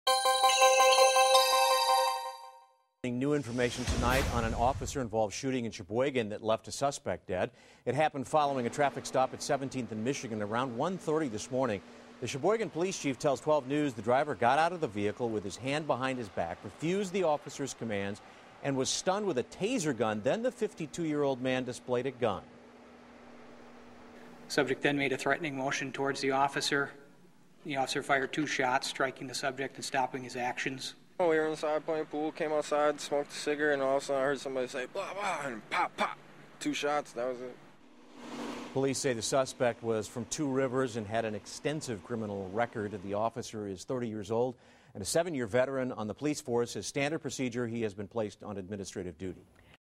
Police: Officer Shot, Killed Armed Man In Sheboygan
Sheboygan's police chief says an officer shot and killed a suspect who resisted arrest during a traffic stop early Friday.